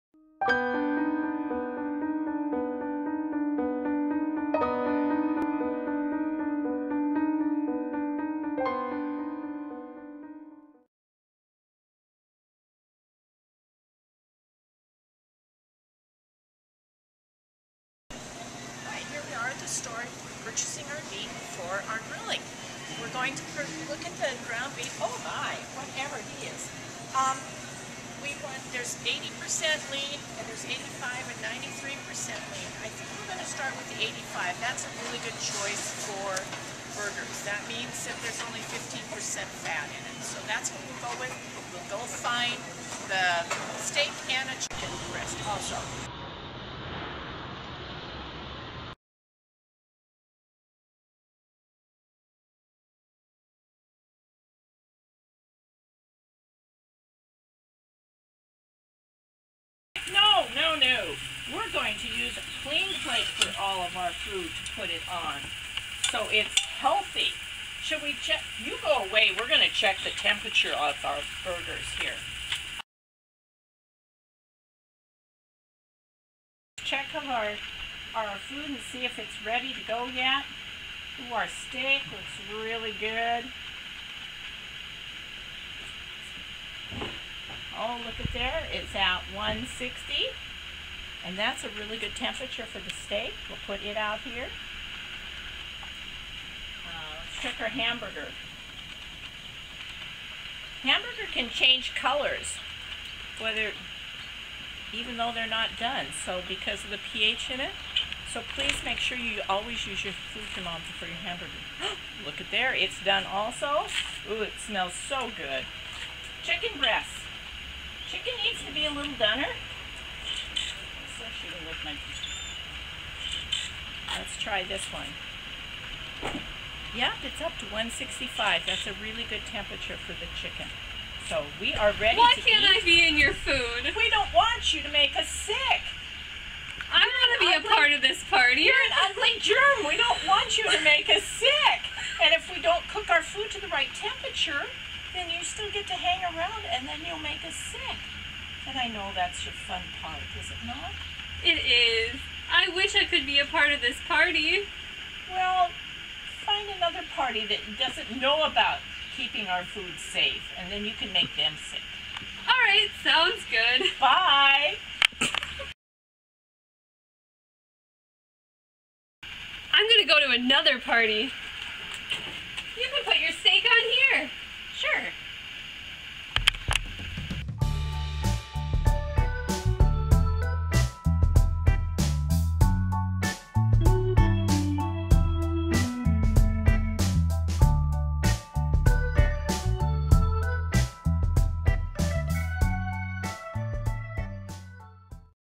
Grilling Food Safely: The Unwanted Guest
Learn how to keep your food safe from bacteria (BAC) by using a food thermometer when grilling.